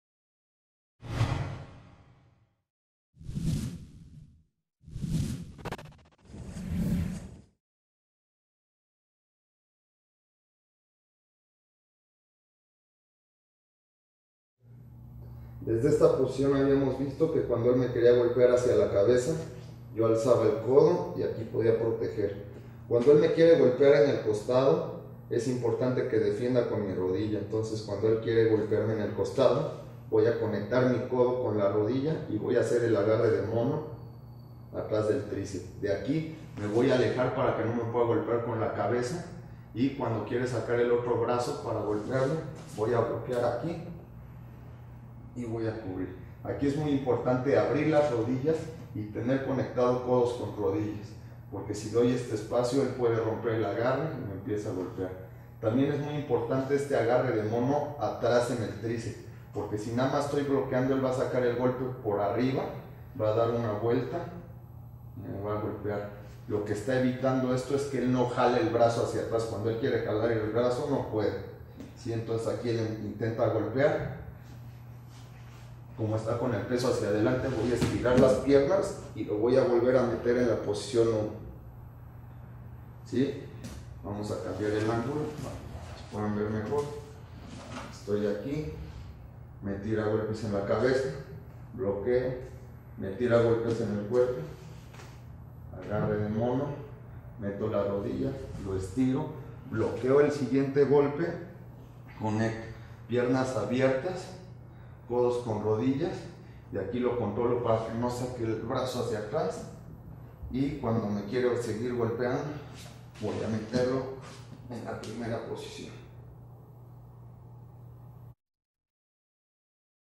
DVD 3 / Posición de Guardia Técnica abriendo la guardia / Mario Delgado
El profesor Mario Delgado, te enseña la Posición de Guardia con la técnica Abriendo la Guardia.